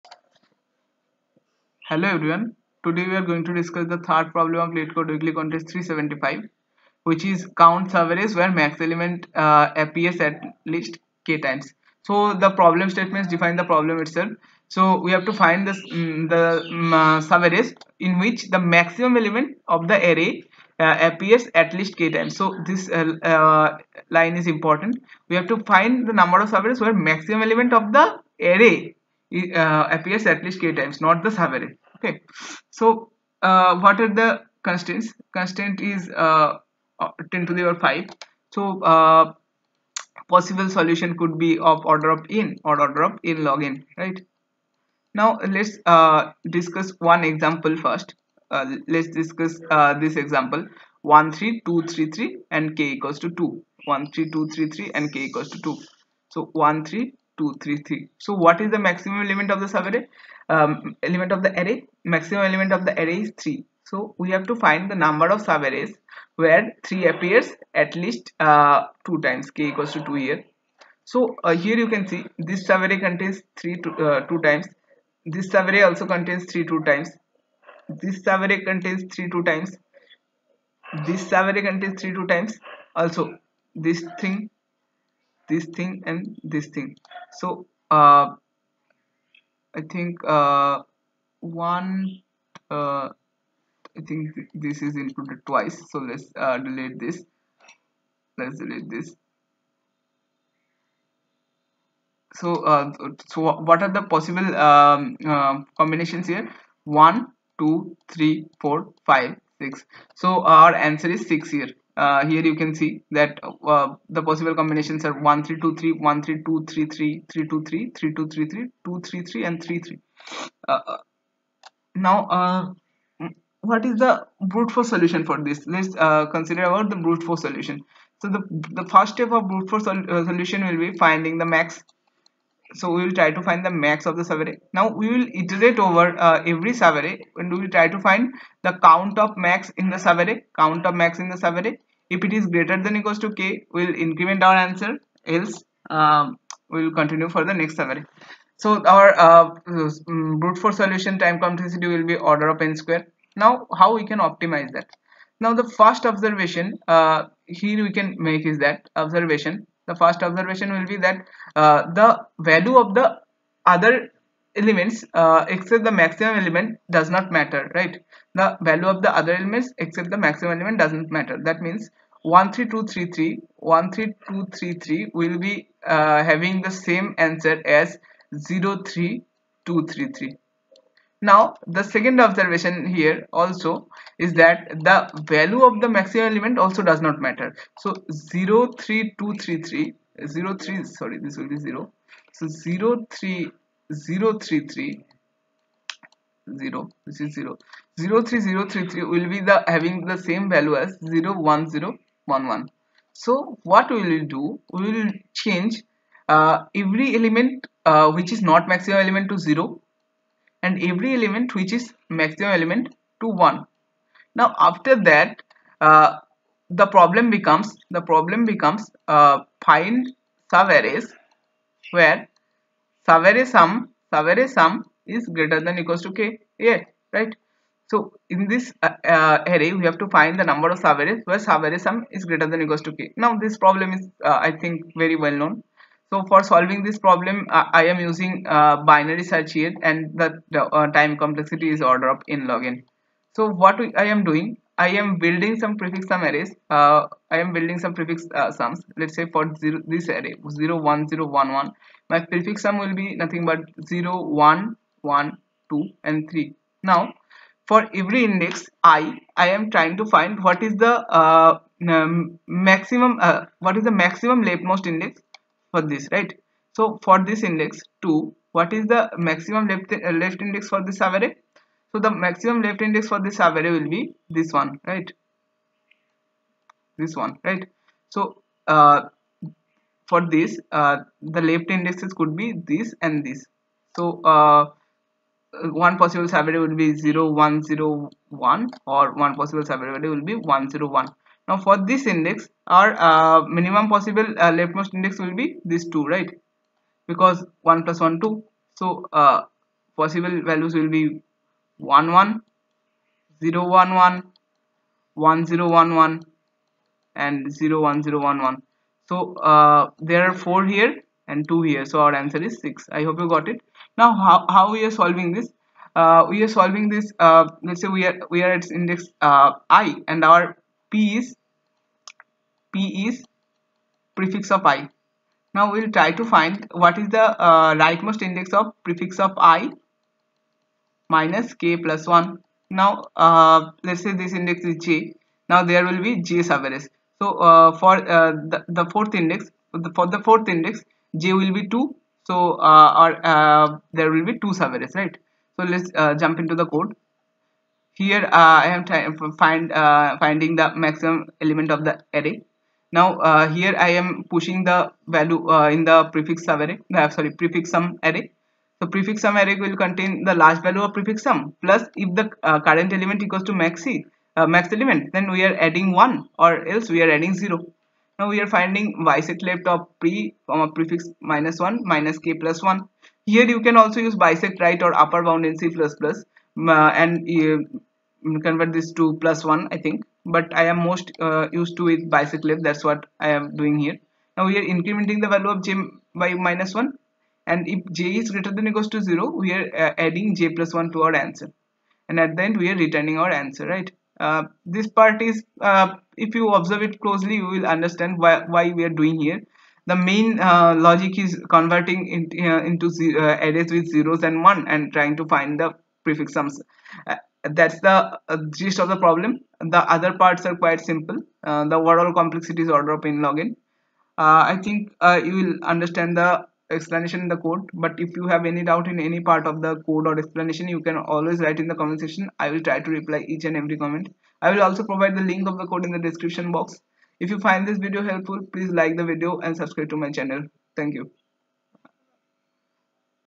Count Subarrays Where Max Element Appears at Least K Times (Leetcode Weekly 375)
Watch in 2X
Here I have provided the solution and explanation of the 3rdproblem of Leetcode Weekly 375